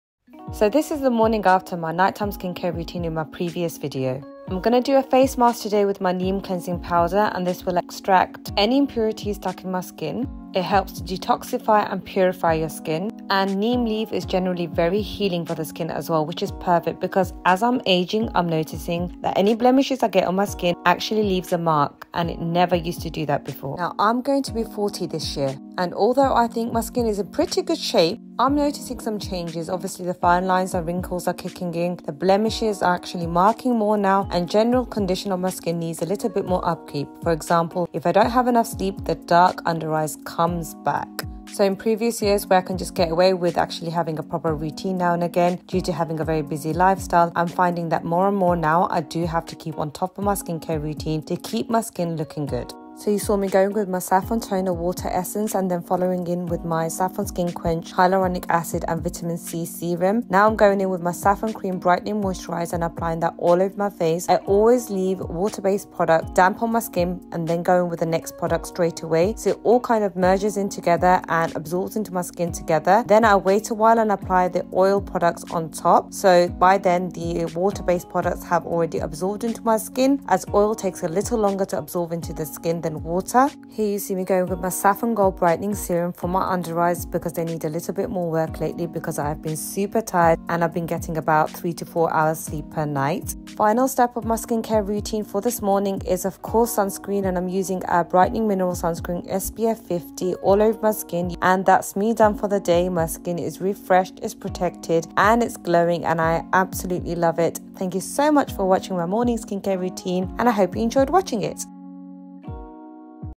𝗠𝗬 𝗗𝗔𝗬 𝗧𝗜𝗠𝗘 𝗥𝗢𝗨𝗧𝗜𝗡𝗘 Morning skincare routine essentials! Start with a gentle
𝗠𝗬 𝗗𝗔𝗬 𝗧𝗜𝗠𝗘 𝗥𝗢𝗨𝗧𝗜𝗡𝗘 Morning skincare routine essentials! Start with a gentle cleanser, follow up with a hydrating toner, apply vitamin C serum for glow, moisturize, and SPF to protect! ☀️✨